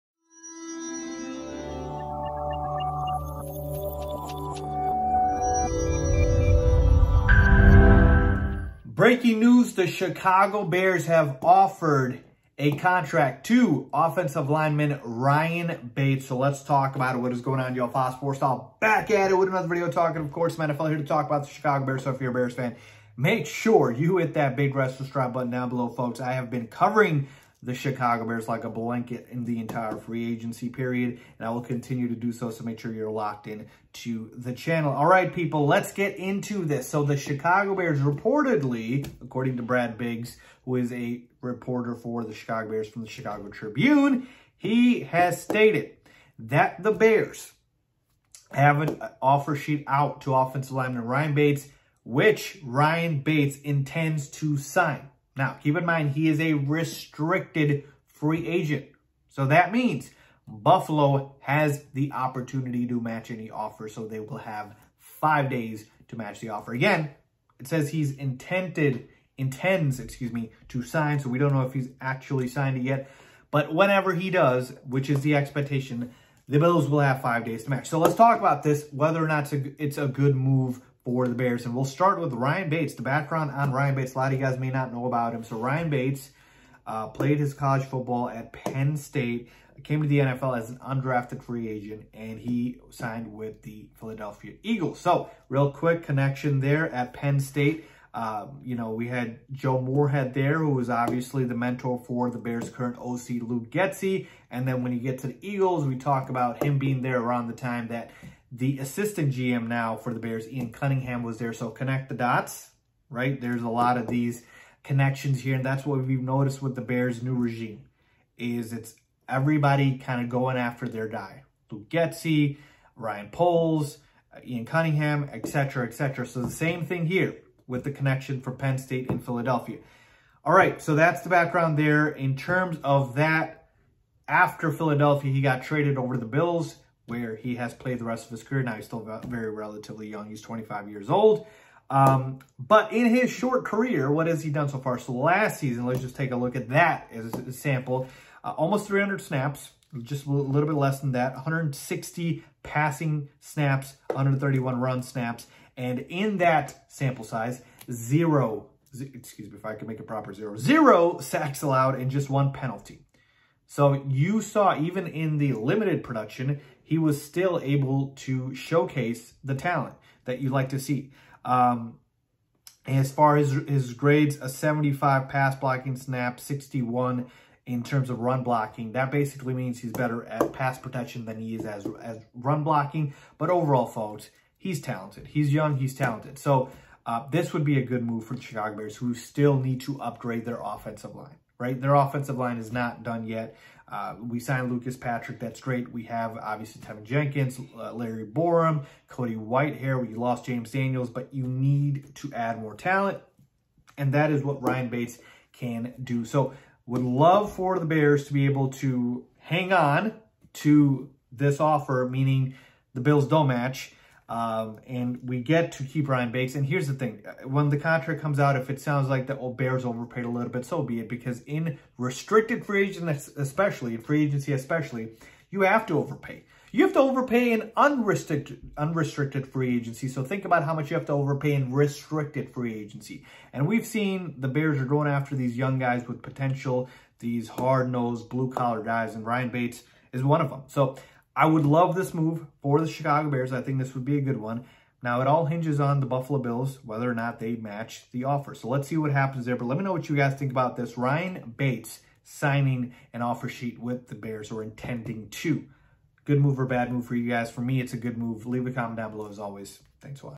Chicago Bears Sign OL Ryan Bates To An Offer Sheet!
In today's video, I give you guys my thoughts on the Bears signing Ryan Bates to an offer sheet.